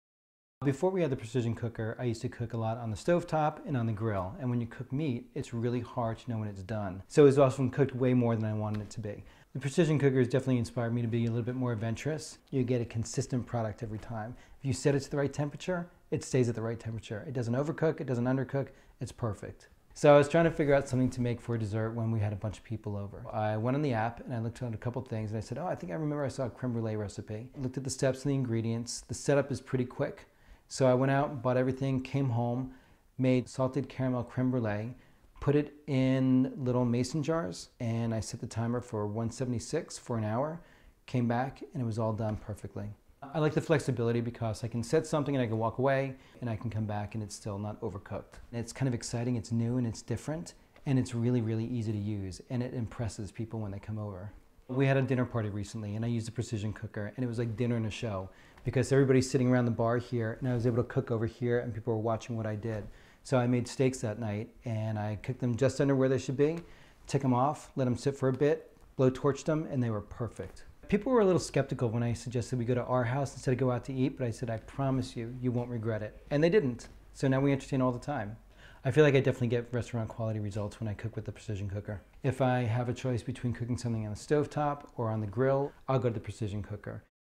anovafoodnerd Danny Della Lana
Meet anovafoodnerd Danny Della Lana and learn how Anova has changed his life in the kitchen and made dinner parties a breeze.